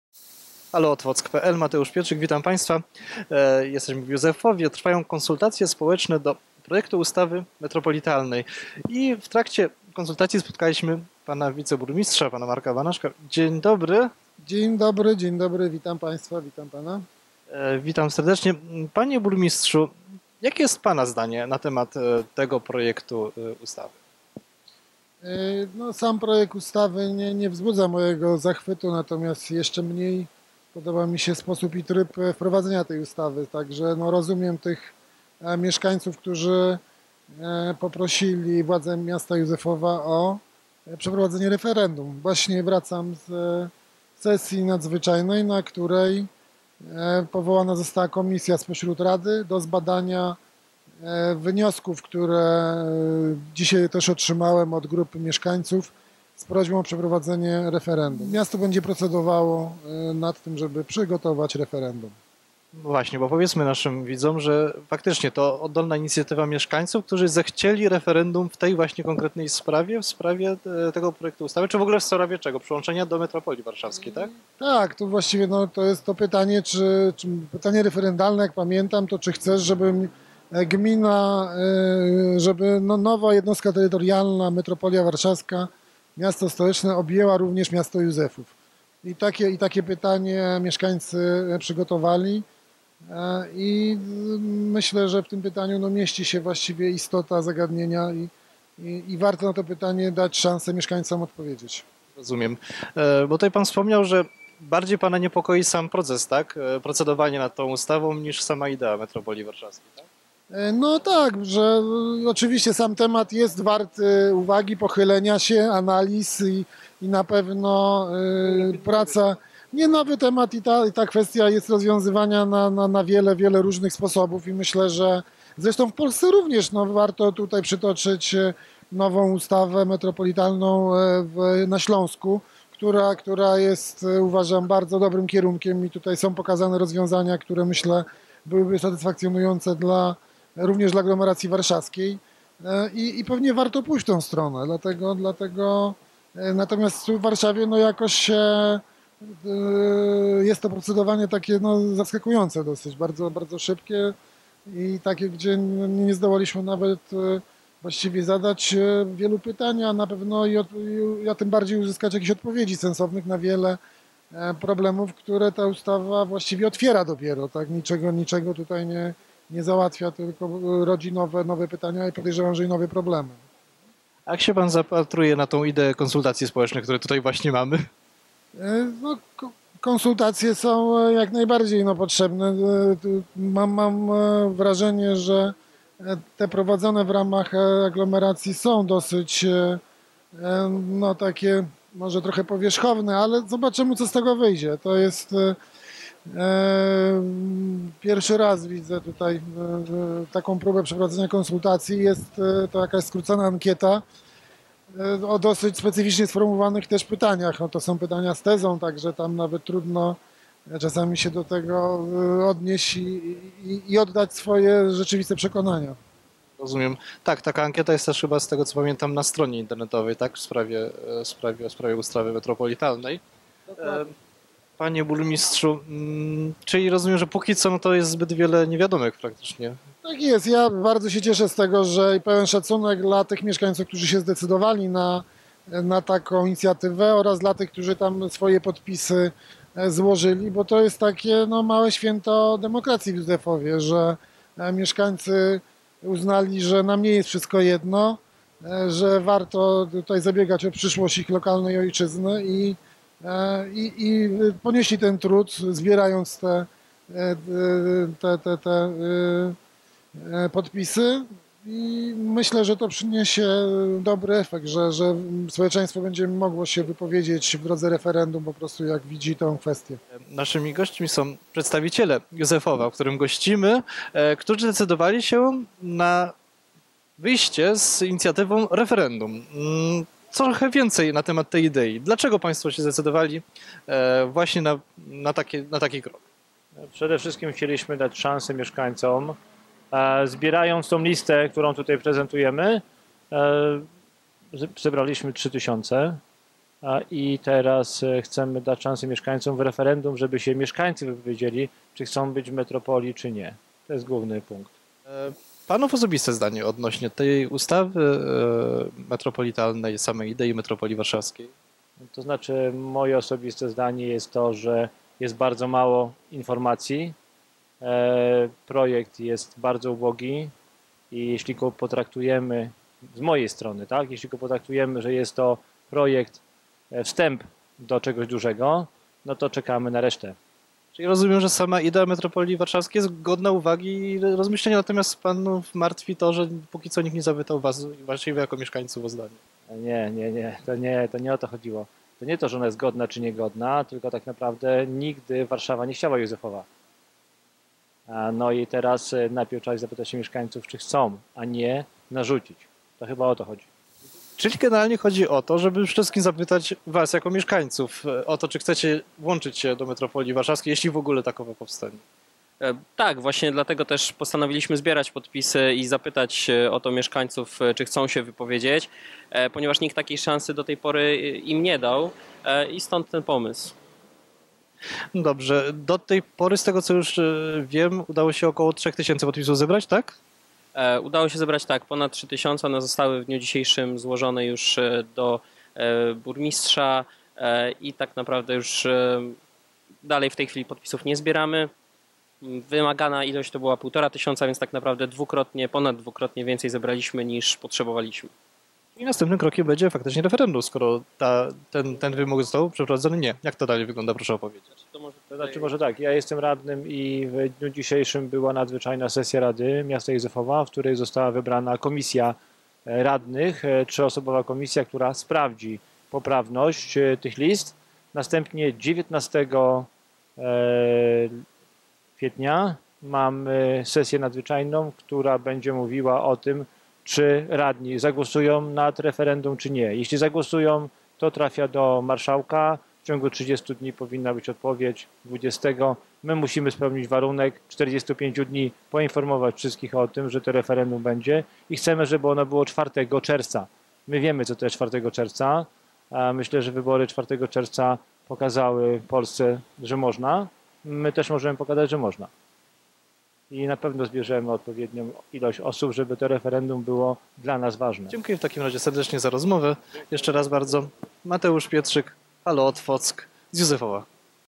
Referendum w Józefowie
Z inicjatywy mieszkańców Józefowa zebrane zostały podpisy pod wnioskiem o przeprowadzenie referendum w sprawie ustawy o metropolii warszawskiej. Czy referendum się odbędzie? Rozmawiamy o tym z wiceburmistrzem Józefowa Markiem Banaszkiem i inicjatorami józefowskiego referendum.